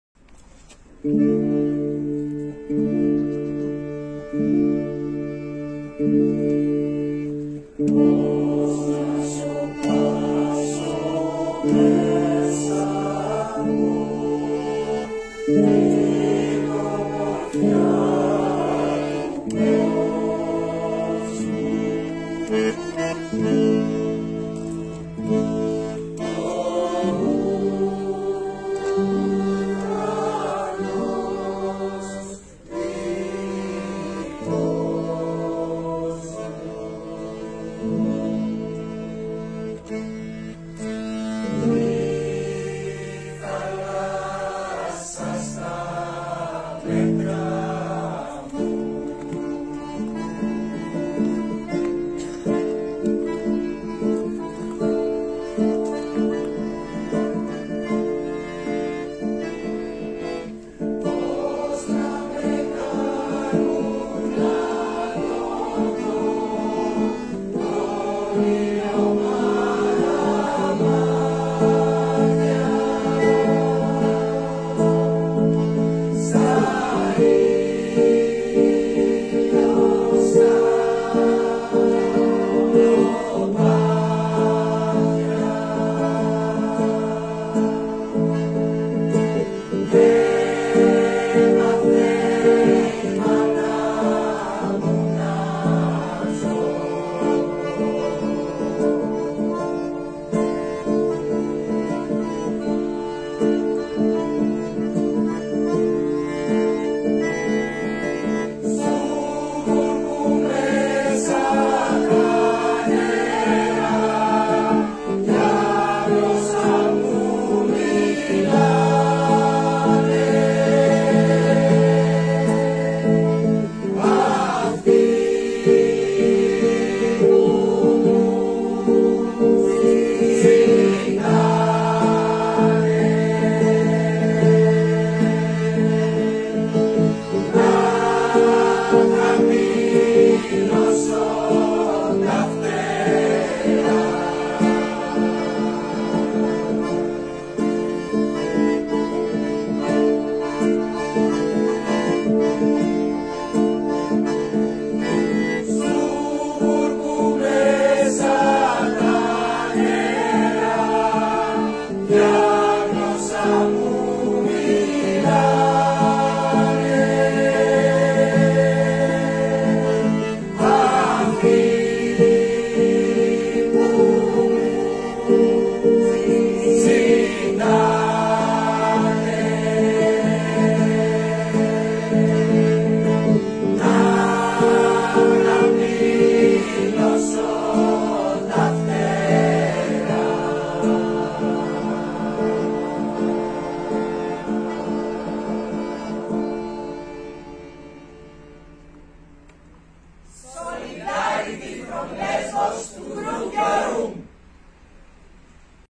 Midilli Cantalaloun Korosu’ndan İhsan Cibelik için Özgürlük Videosu
Midillli Enternasyonal Sanat Cephesi üyeleri 23 Mayıs Pazartesi günü Midilli'de bir araya gelerek 17 Mayıs'ta Almanya'da tutuklanan Grup Yorum Emekçisi İhsan Cibelik için bir araya geldiler.
Enternasyonal Sanat Cephesi